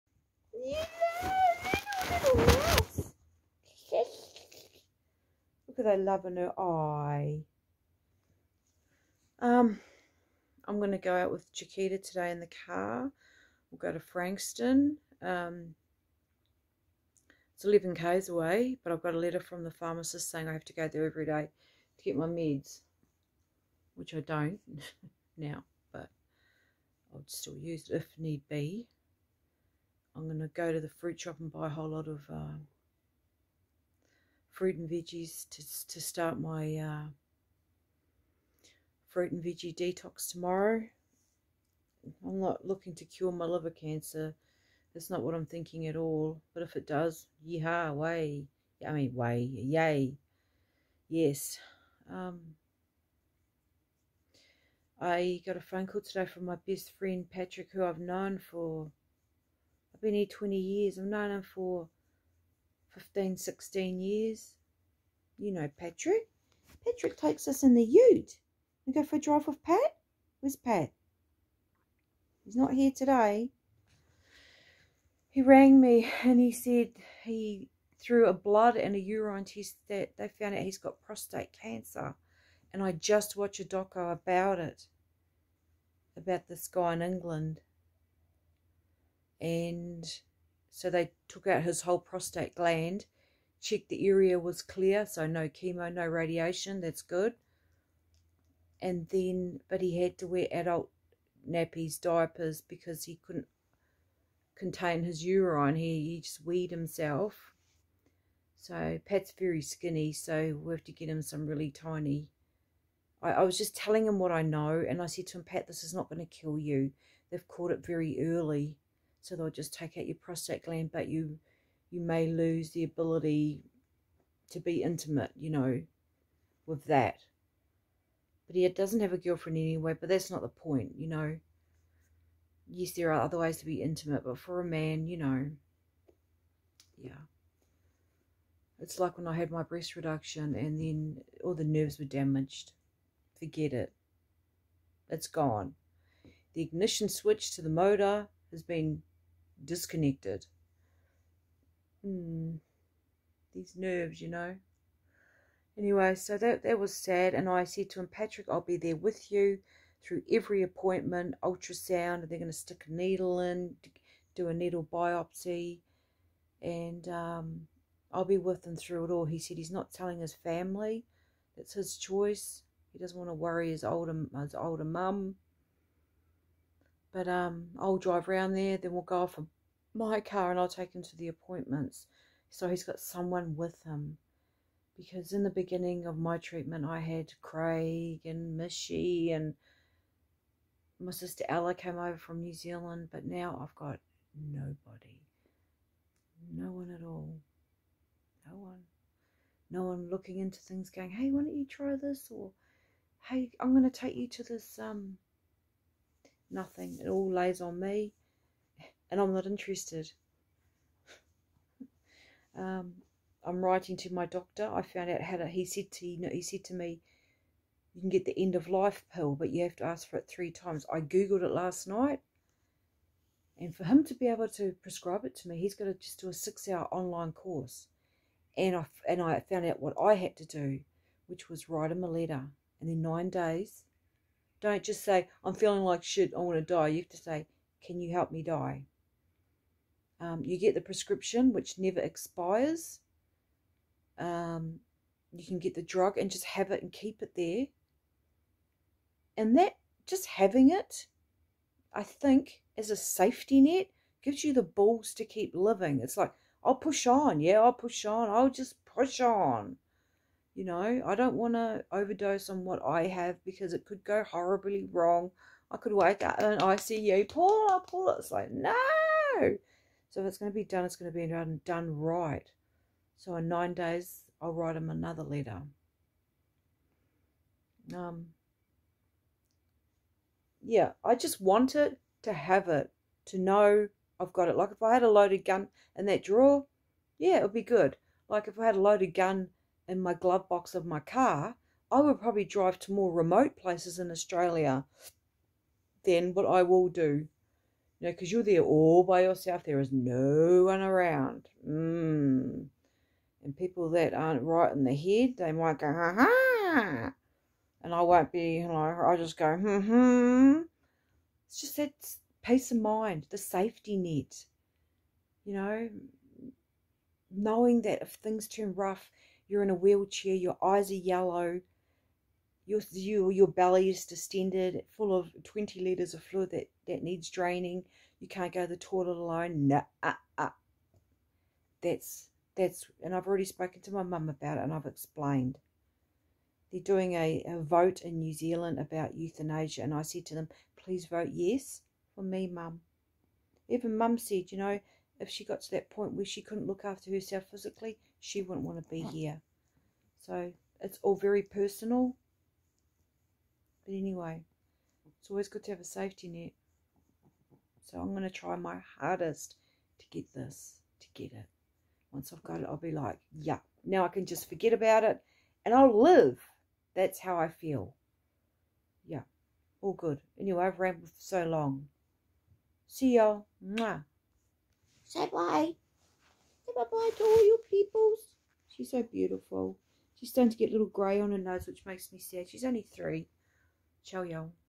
End of life pill,,highly personal choice,💊💉⚰My friend has prostate cancer😪
Im starting the process of getting the script for the end of life drug. I found out what I have to do so wrote letter number one. Im willing to fight for it if i have to & even go to the media if denied...I need that safety net. I believe having that safety net,peace of mind. I will forget I have cancer,stop worrying & let it do its thing...I want to be the girl I used to be...full of energy,laughing,making others laugh..being strong mentally & physically...i guess its like walking the dark street with a loaded gun in ur pocket. Im hoping you would feel safer. I want a good death. Peaceful,painfree, I will not accept laying in a hospice turning to bones in front of my dearest parents & family.